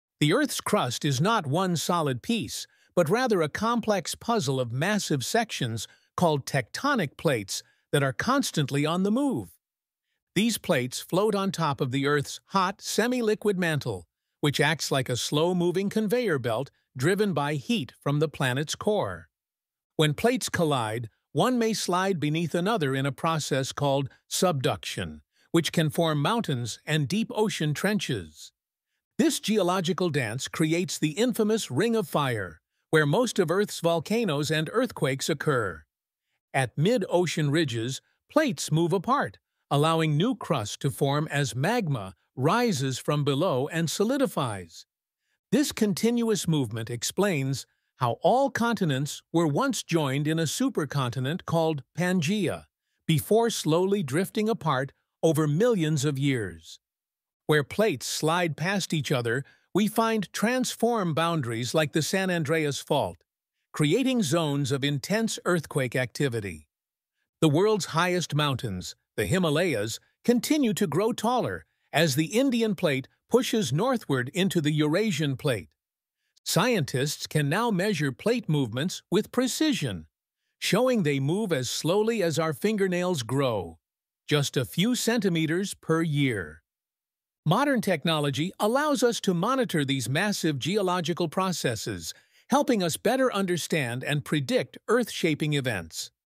The Earth's Tectonic Dance: How Our Planet Changes Shape
🌍 Dive into the fascinating world of tectonic plates and discover how they shape our planet! From the formation of mountains to the dramatic Ring of Fire, learn how the Earth's crust is a dynamic puzzle constantly at work. 🌋 Join us as we explore the layers of the Earth, the creation of new land, and how scientists track these movements in real-time. Don't miss out on this incredible journey beneath our feet!